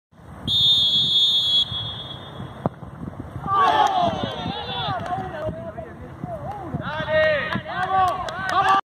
Gol de penal de Esteban Brizuela - Argentino
LMF - Fecha16. 4ta categoría. Rivadavia 3-3 Argentino